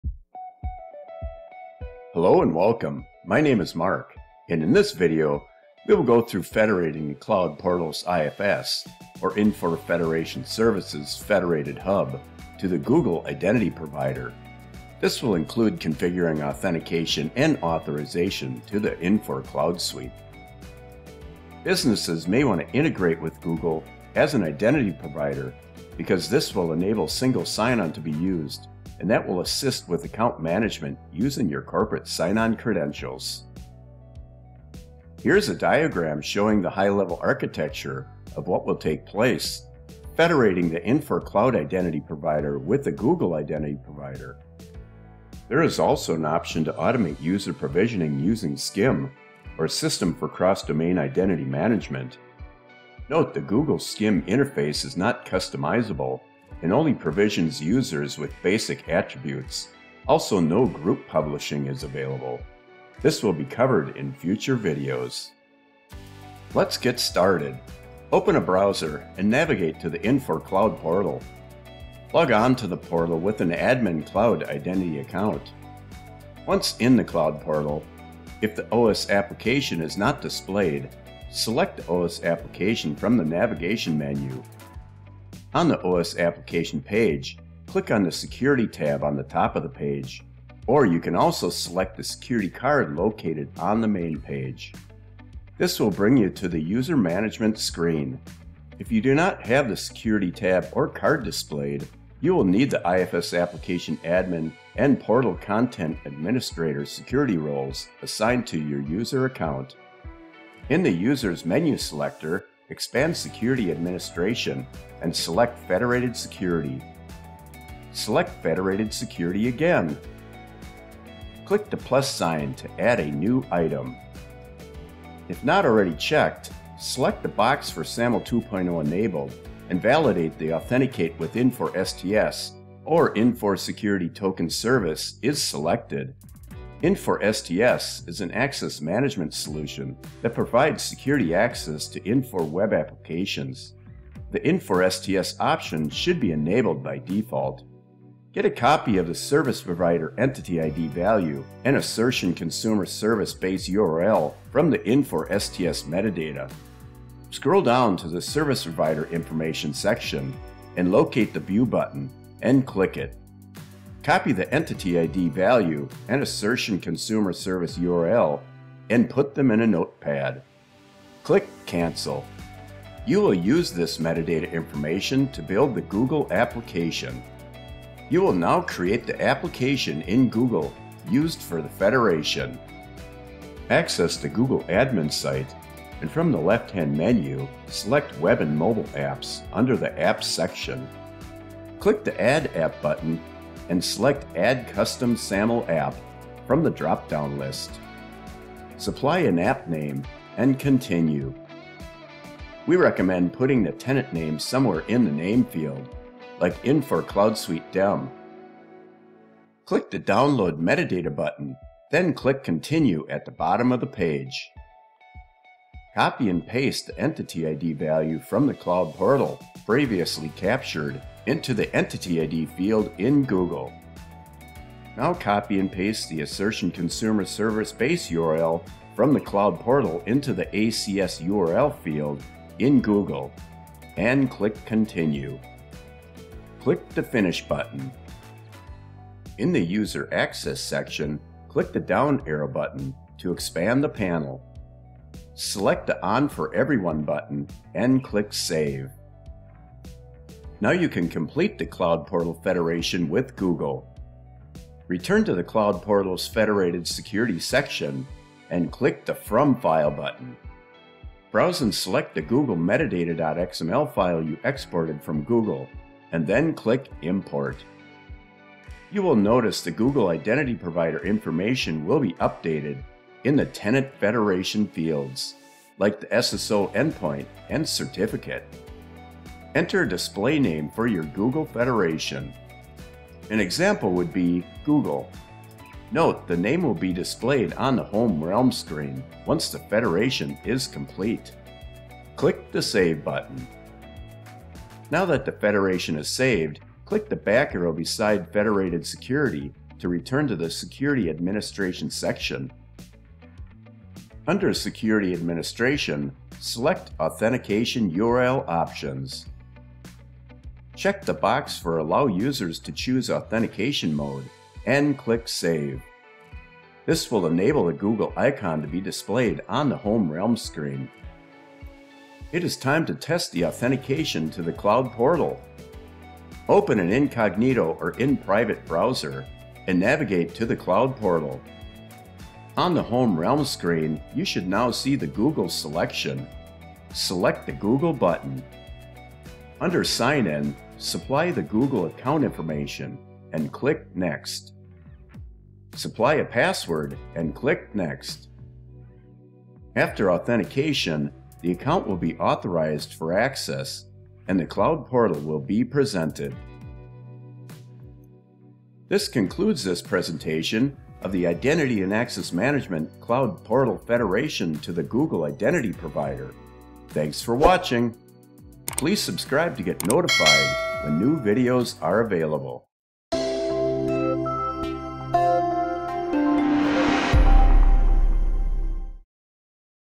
Using Cloud SSO with Google Identity Provider and SAML Federation
This video shows how to implement Cloud SAML Federation with Google Identity Provider for seamless single sign-on to your Infor Cloud resources. Users can effortlessly log in to various cloud services with their Google credentials, enhancing user experience and security.

Title: 00:00
Agenda/Outline: 00:05
Benefits of performing SAML Federation: 00:21
High Level Architecture Diagram 00:34
Gather Cloud Metadata Information 01:06
Google Identity Provider federation Setup 03:03
OneLogin Application Details Setup 03:51
Google Metadata Export 03:29
Google User Access 03:59
Cloud Federation Setup 04:14
Test OneLogin Authentication 05:26
Outro 06:12